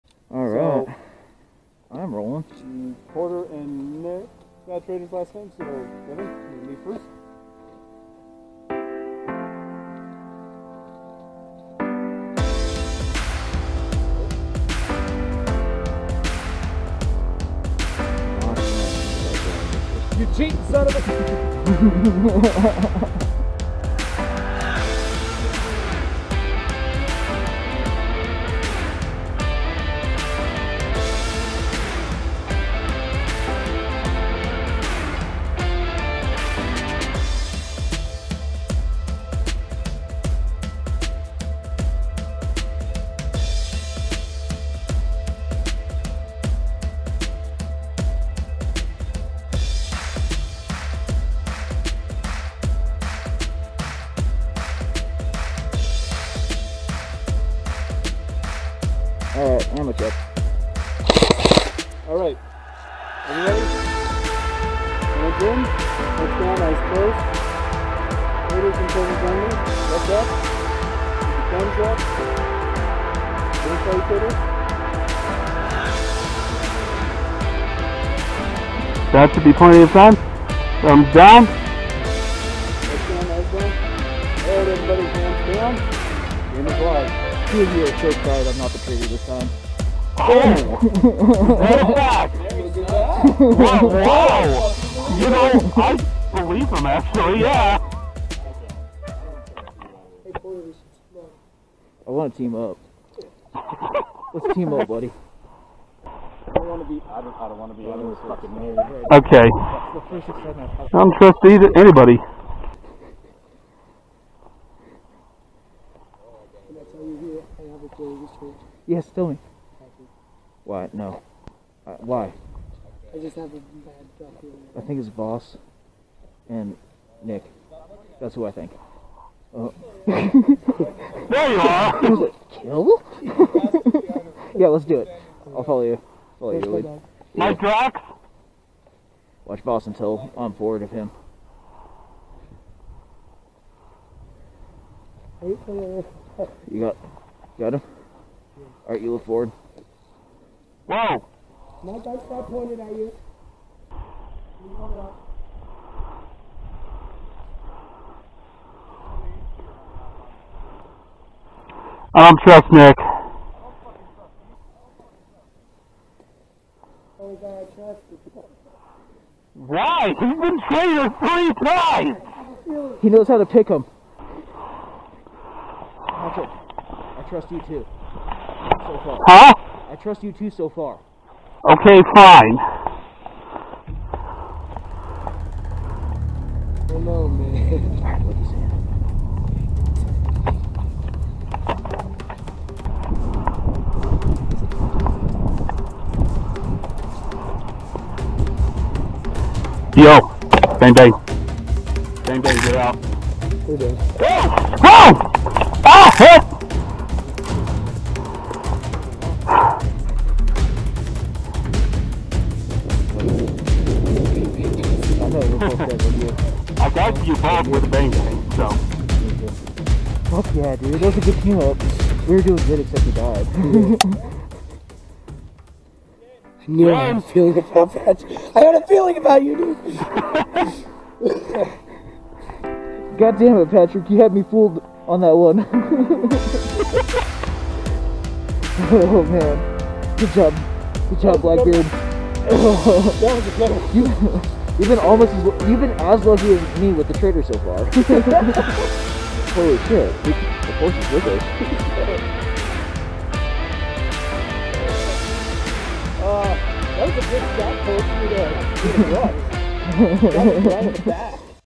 Feb TTT Game 4
Trouble in Terror Town. A Ronin team favorite.
Ronin airsoft of northern Arizona.
Here with us today Is BlackBeard, Master Po
and the son of Chaos the Reverend Porter
as well as some new faces that will hopefully become familiar faces.